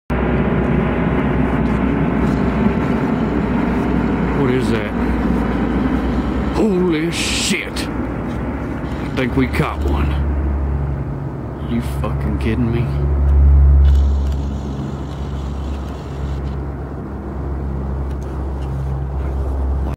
UFO SIGHTING IN MISSOURI 3-31-11
I spoted this in the hills of missouri and by the video you can tell i'm shocked, I had been seing planes flying over but this was no plane .